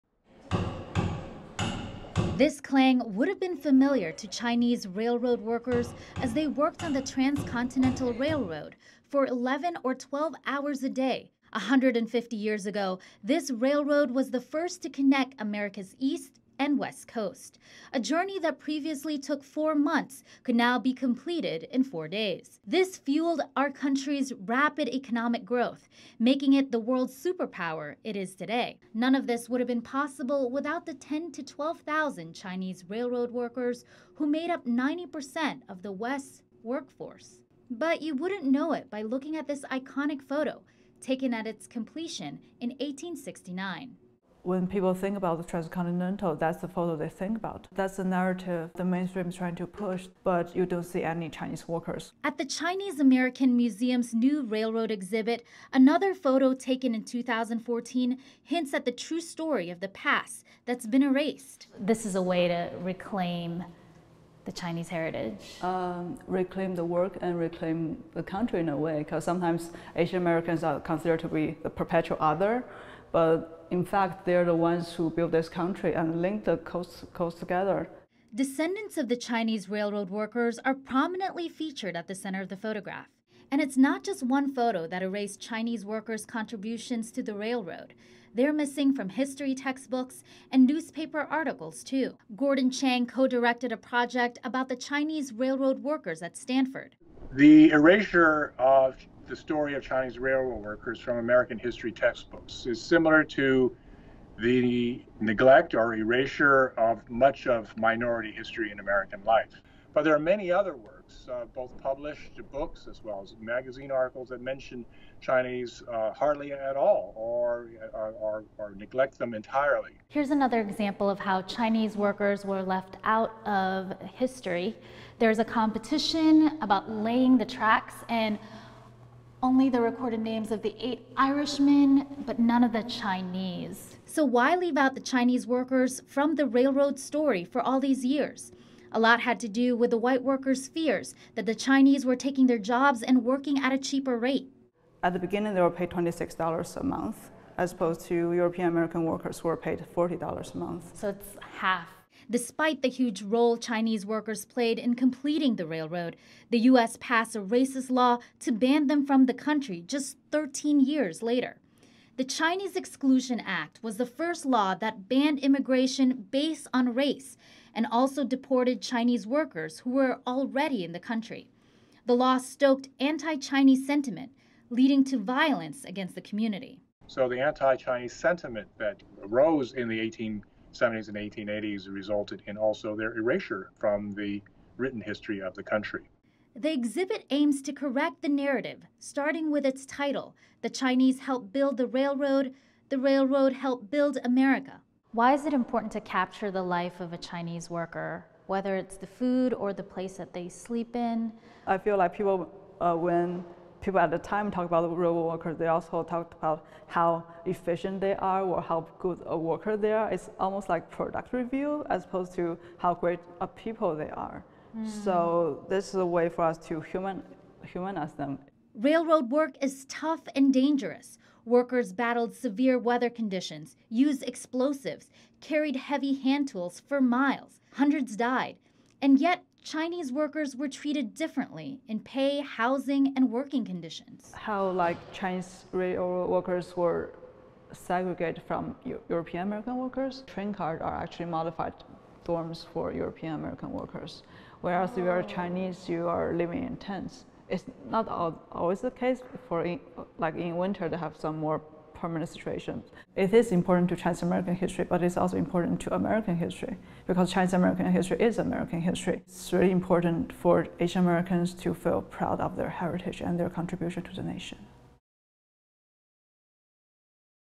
Remembering Chinese railroad workers
150 years after the completion of the First Transcontinental Railroad, a local Chicago Museum highlights Chinese workers' contributions.